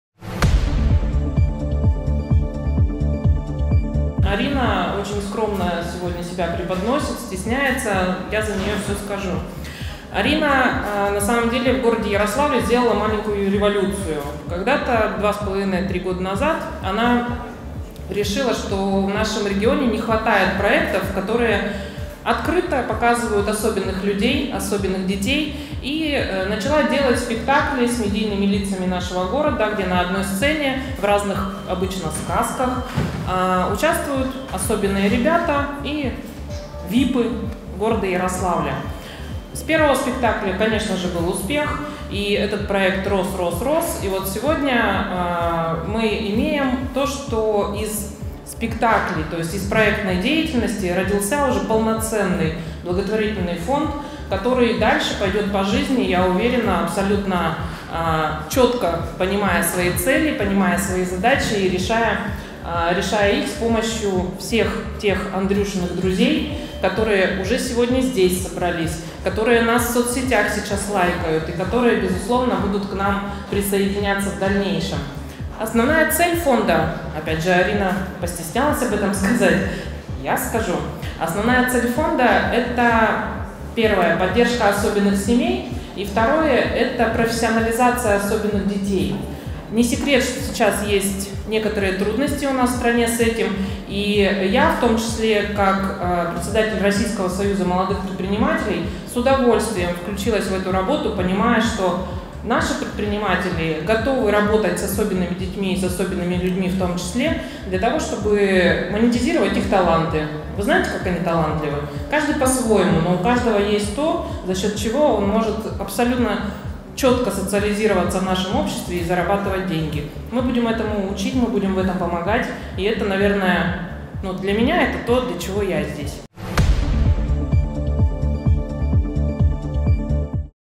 «Наши предприниматели готовы работать с особыми детьми, чтобы раскрыть их таланты» — Елена Мильто
Член Комиссии ОП РФ по развитию экономики, предпринимательства, сферы услуг и потребительского рынка Елена Мильто в рамках торжественного открытия фотовыставки «Семейные истории особенных людей (люди с ОВЗ)», ОП РФ, 3 июля 2019 года